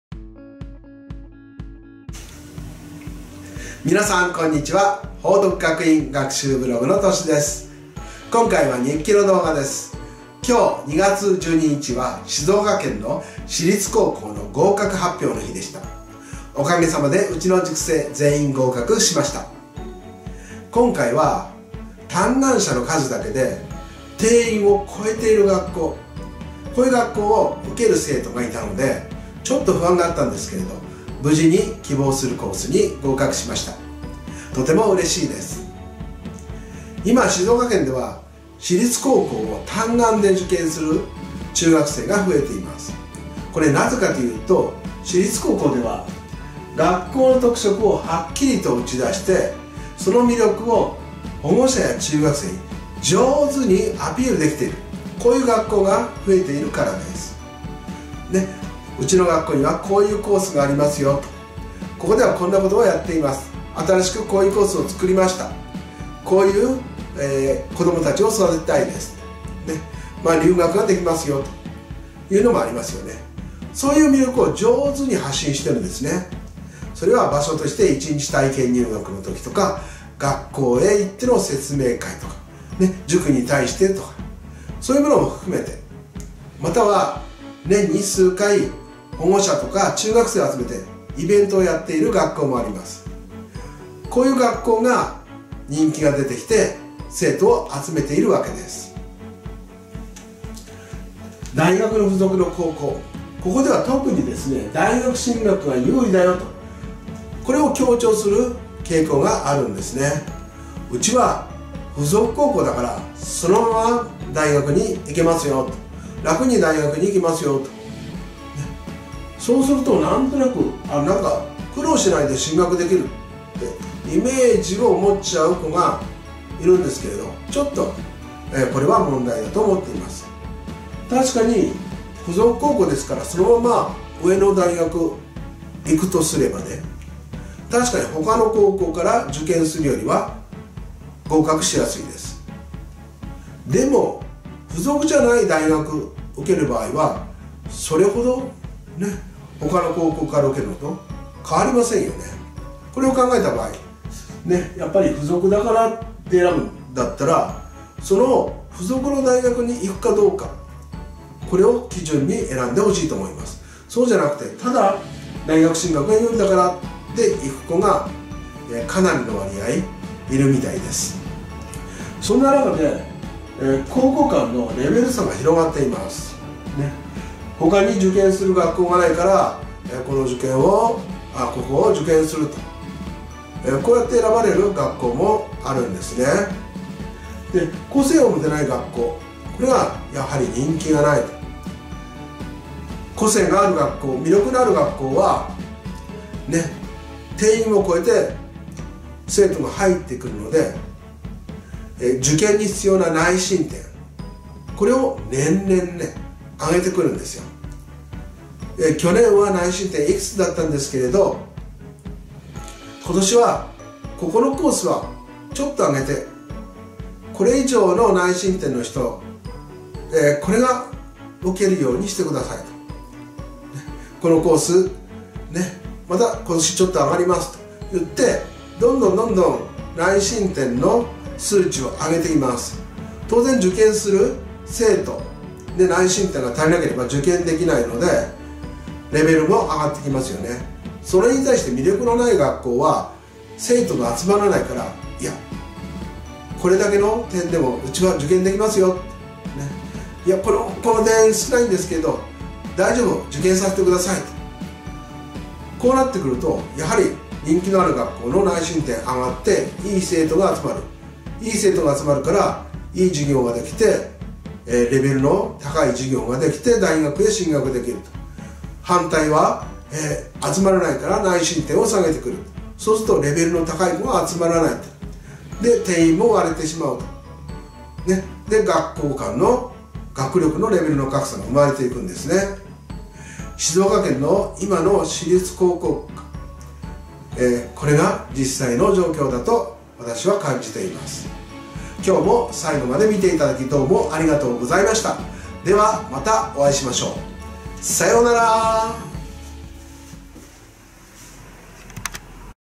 静岡県私立高校合格発表 【気ままな日記】
今日2月12日は、静岡県の私立高校の合格発表の日でした。